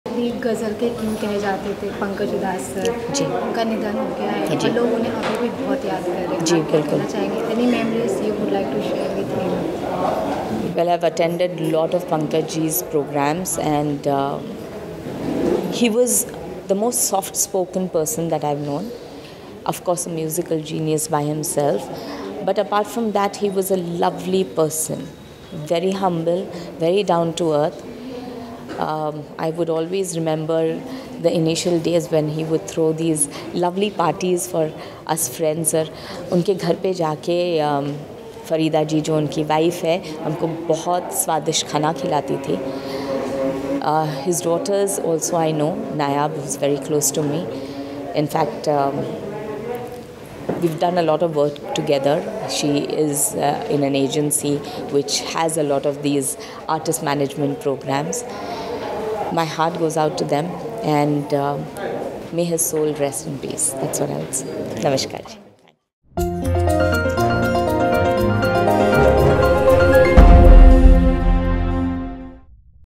Bhagyashree ने गायक Pankaj Udhas को ऐसे किया याद
फिल्म अभिनेत्री भाग्यश्री ने पंकज उधास के दुखद निधन पर प्रतिक्रिया जाहिर करते हुए कहा कि वो पंकज जी के काफी करीब थी। पंकज उधास के निधन से वो काफी आहत हैं।